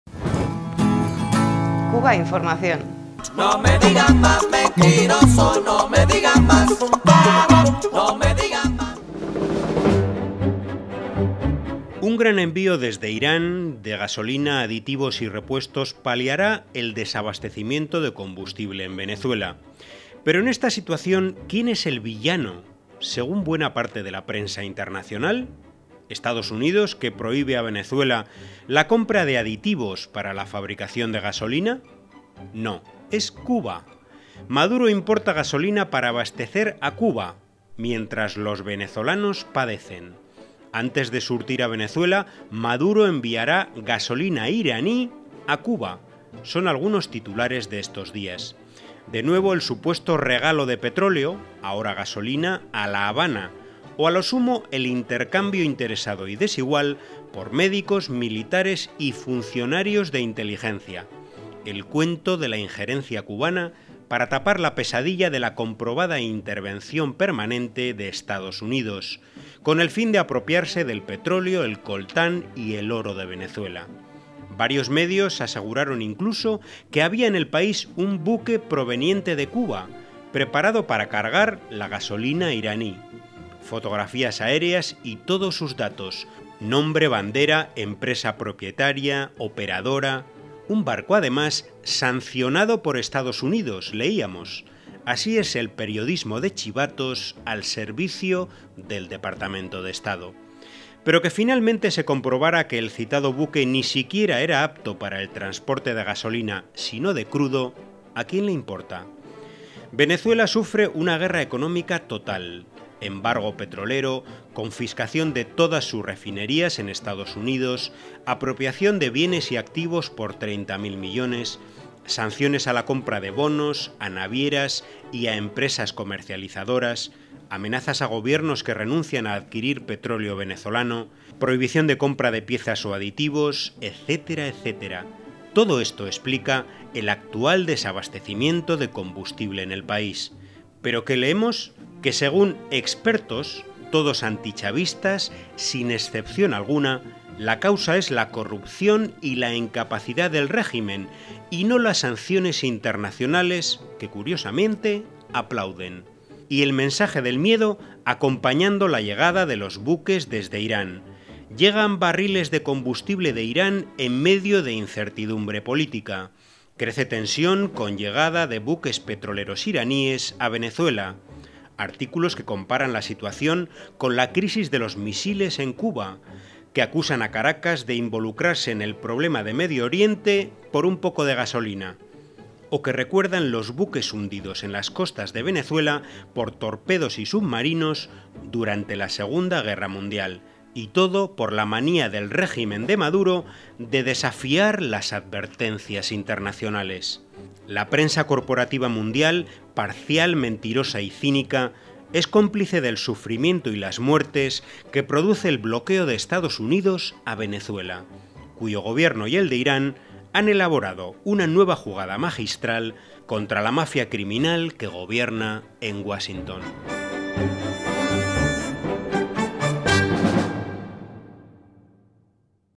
Gasolina iraní para Cuba y periodismo de chivatos
(+Italiano/Français/ Deutsch)

Benzina iraniana per Cuba e giornalismo di delazione
L’essence iranienne pour Cuba et le journalisme des cafards
Iranisches Benzin für Kuba und der Journalismus der Petzer

Un gran envío, desde Irán, de gasolina, aditivos y repuestos paliará el desabastecimiento de combustible en Venezuela. Pero en esta situación, ¿quién es el “villano”, según buena parte de la prensa internacional? ¿EEUU, que prohíbe a Venezuela la compra de aditivos para la fabricación de gasolina? No: es Cuba.